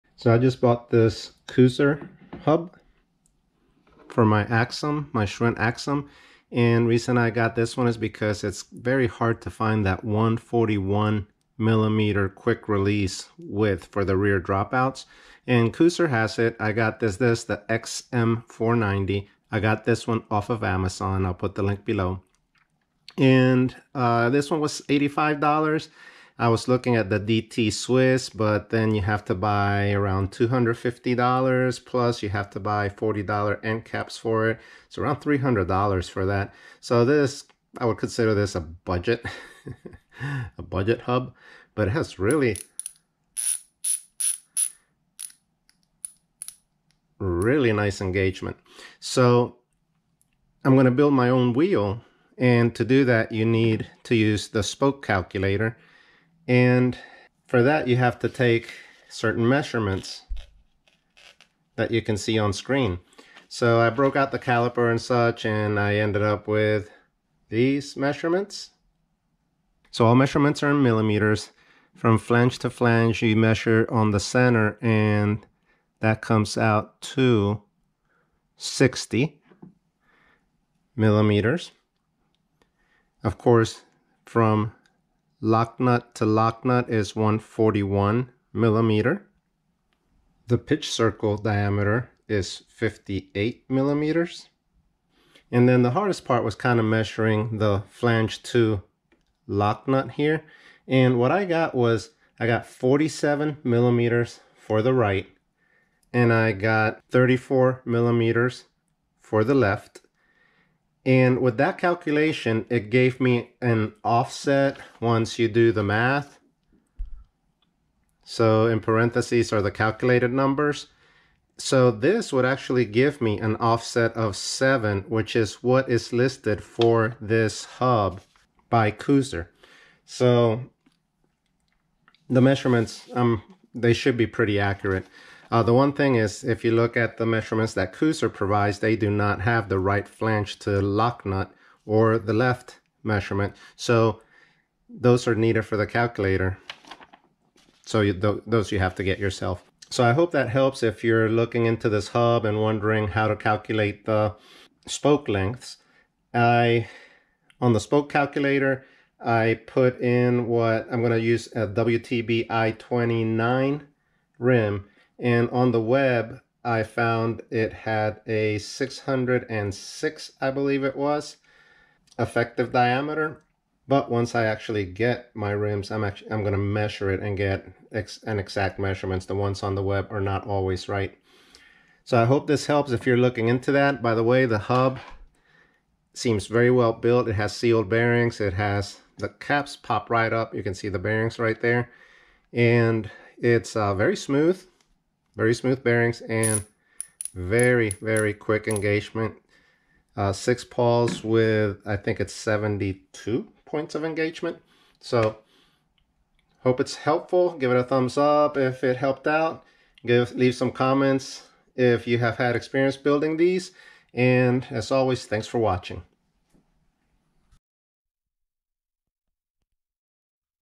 Koozer XM490 141mm QR Hub Specs and Measurements for Spoke Calculations
The Koozer XM490 is one of the few 141mm QR Boost hubs available for bikes such as the Schwinn Axum and the Trek Roscoe which both use this uncommon rear wheel spacing.
Koozer 141mm QR Hub
Digital caliper
Measurements:
OLD = 141mm
PCD = 58mm
Flange to flange = 61mm measured (not 60 like I say in the video)
Left flange to locknut = 34mm
Right flange to locknut = 47mm
Right flange to center = 36.5mm
Left flange to center = 23.5mm
*I recommend you verify these measurements with your hub and am not responsible for any errors.